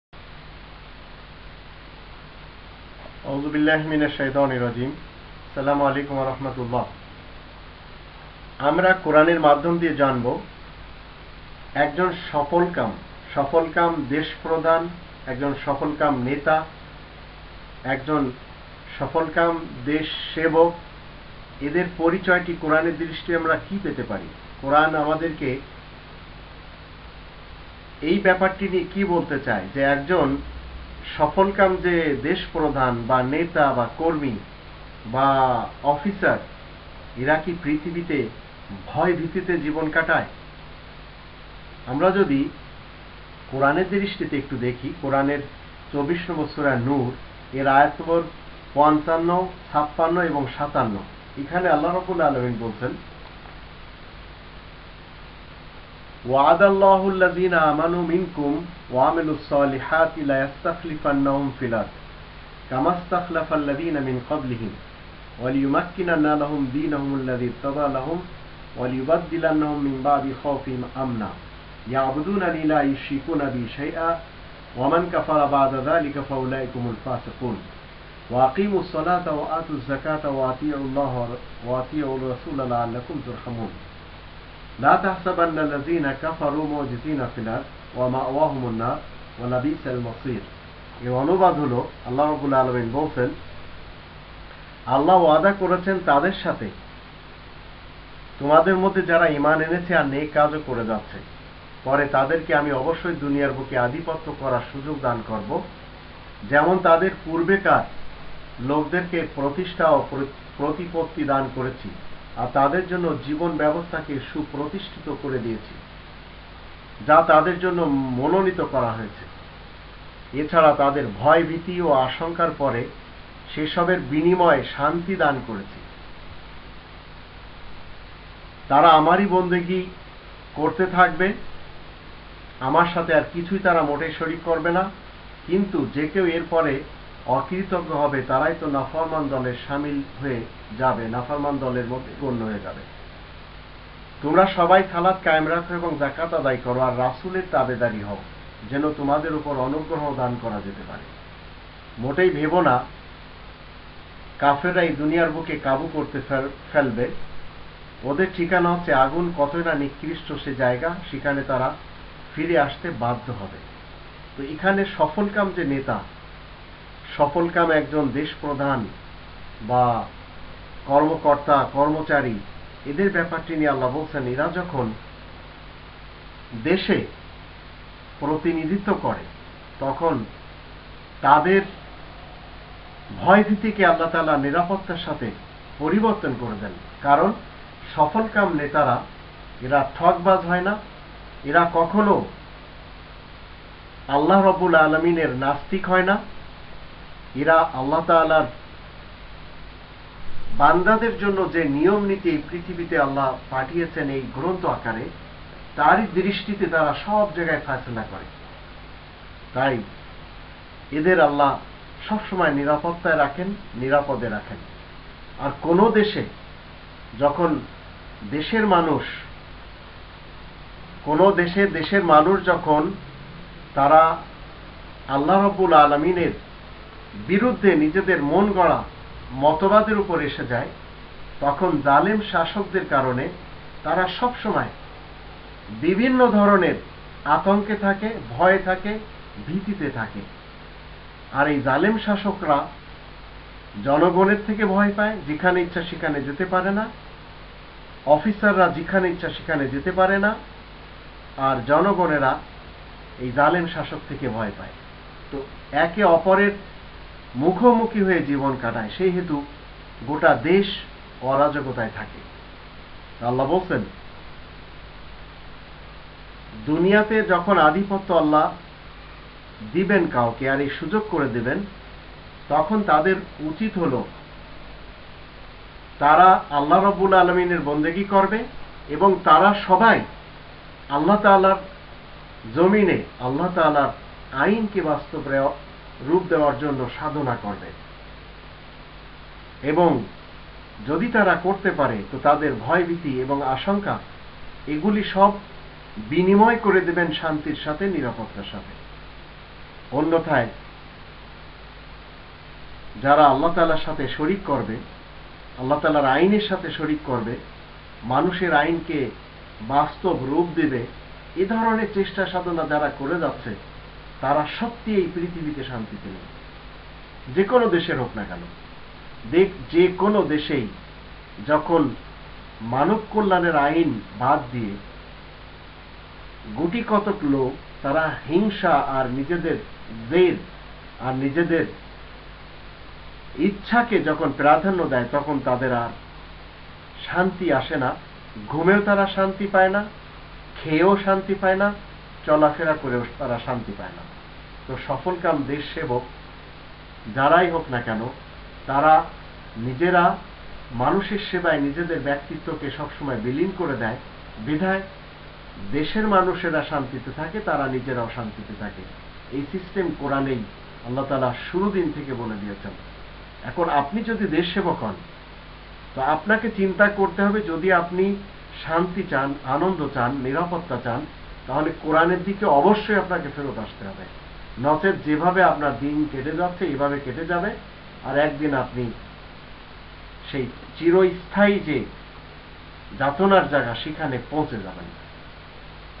কুরআন এবং সফলকাম দেশ প্রধান ও নেতা কর্মীদের পরিচয়
DR.A,Q.M MAHBOOBULLAH JESSORY
Chairman;Bangladeshi Seba Party